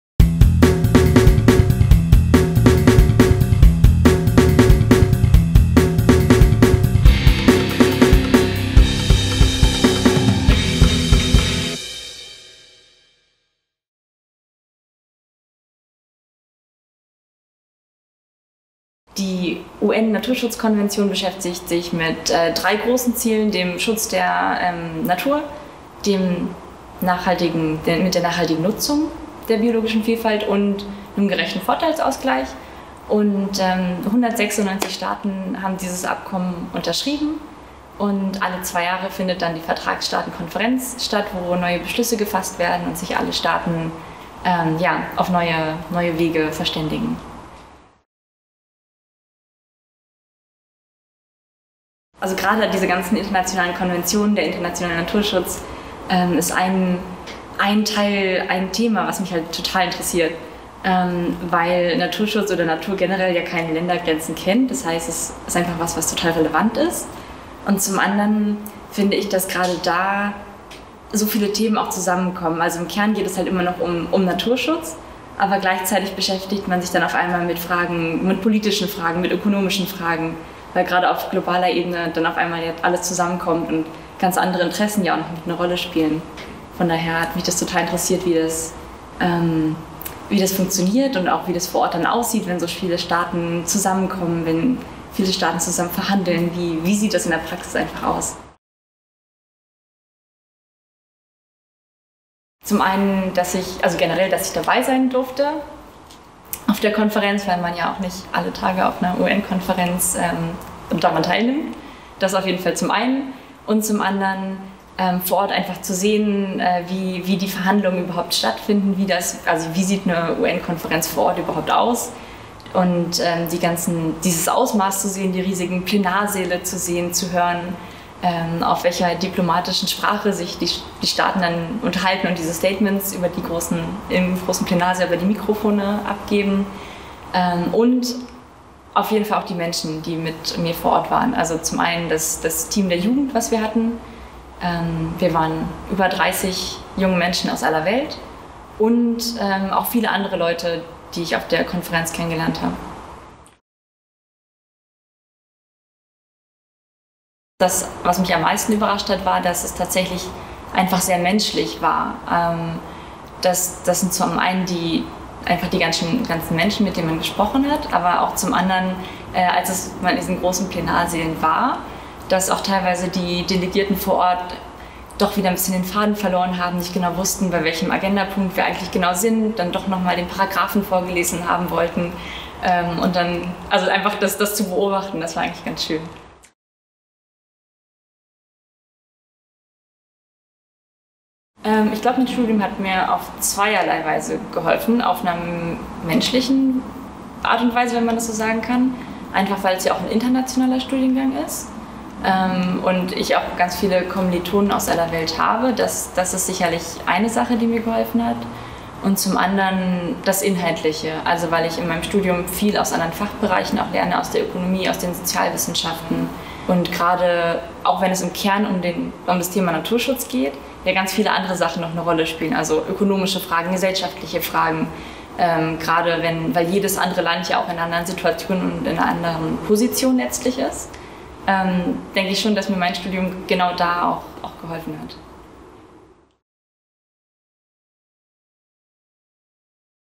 Auf ein Wort mit Adina Arth, Masterstudierende an der CAU
Adina Arth studiert an der UniKiel den Master Sustainability, Society and the Environment. 2018 war sie Teil der deutschen Jugenddelegation auf der UNNaturschutzkonferenz. Wir haben Adina im Botanischen Garten der CAU auf ein Wort getroffen und gefragt, wie ihr die Biodiversitätskonferenz gefallen hat.